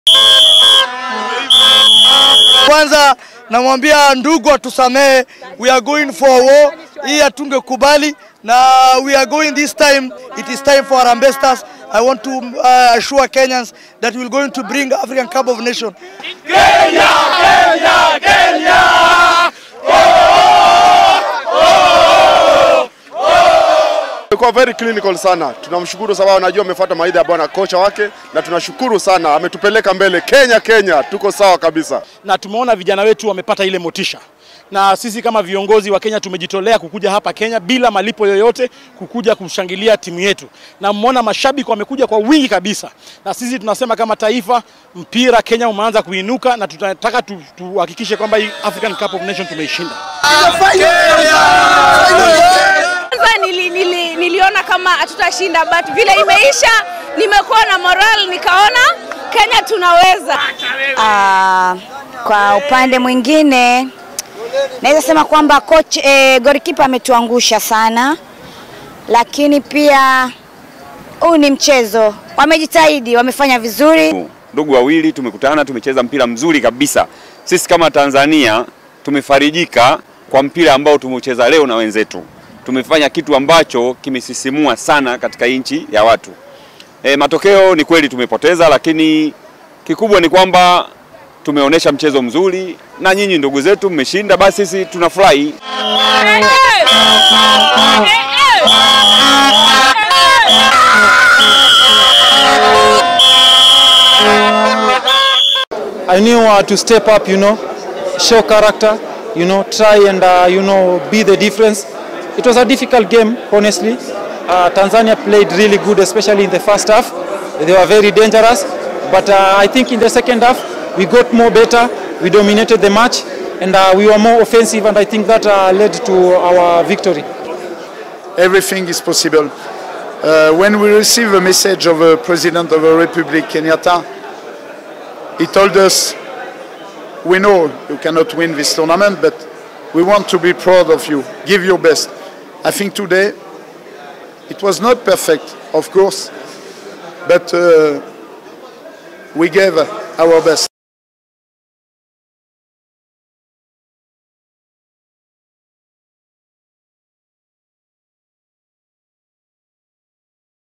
Kenyans celebrate after Harambee Stars beat Tanzania in AFCON 2019
Kenyans celebrate after Harambee Stars beat Tanzania Team in Egypt AFCON 2019.